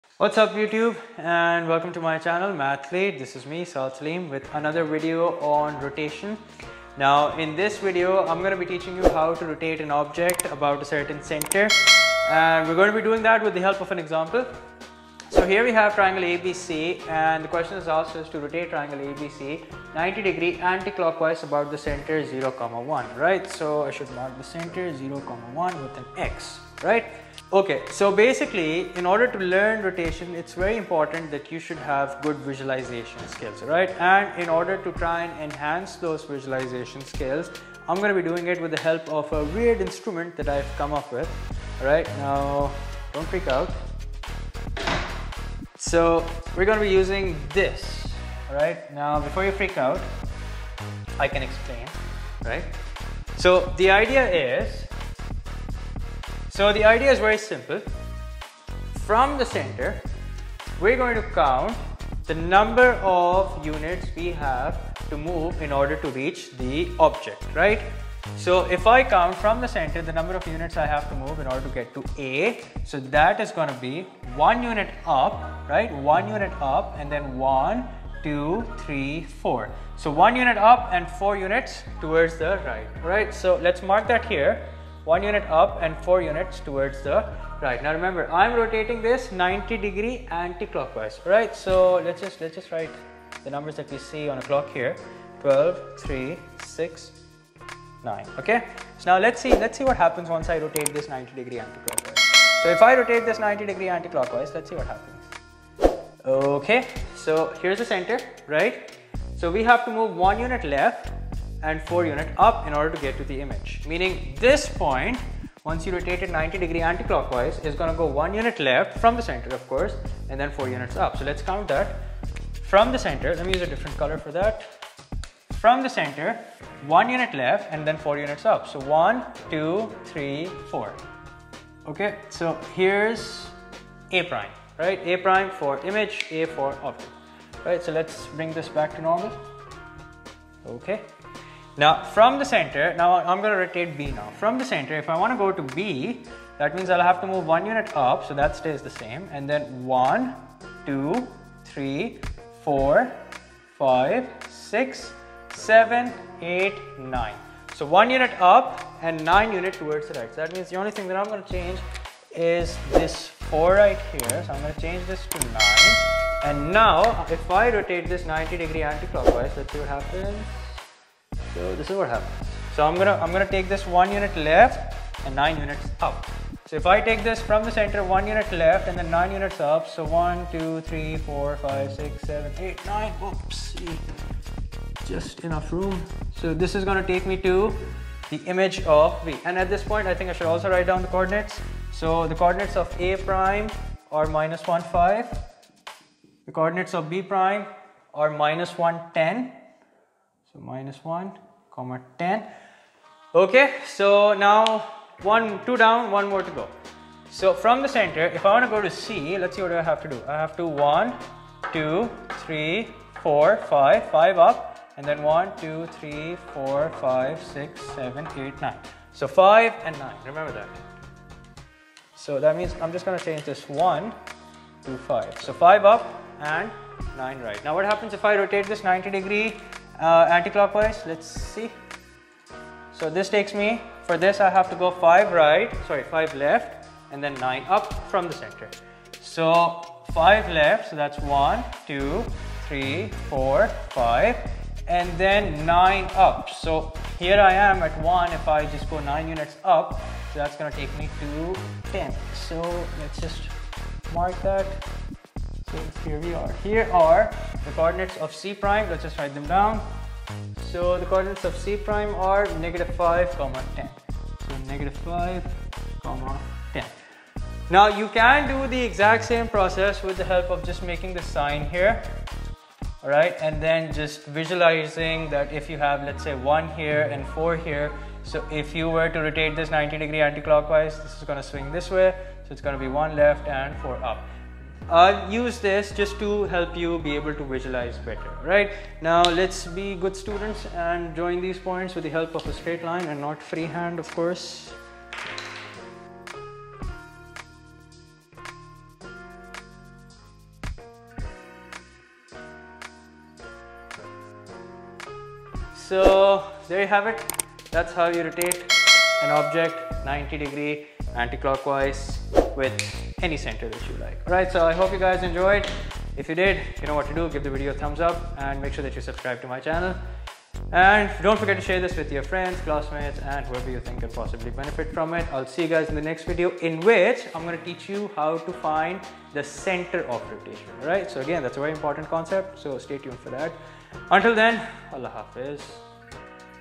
Transformation - Rotation: How to Rotate any object (Easiest Way)
In this video, we will learn how to rotate an object 90 degrees anti-clockwise with the help of a ‘weird tool’ as I mention in the video.
Who am I?
Where do I teach?